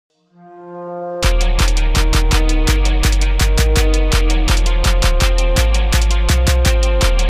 If you win your Super Bowl bet what car are you buying? (comment the car you'll buy)
With the Super Bowl happening today many people are placing big bets on either the Chiefs or Eagles. If you're placing Super Bowl bets and win, what car are you going to buy?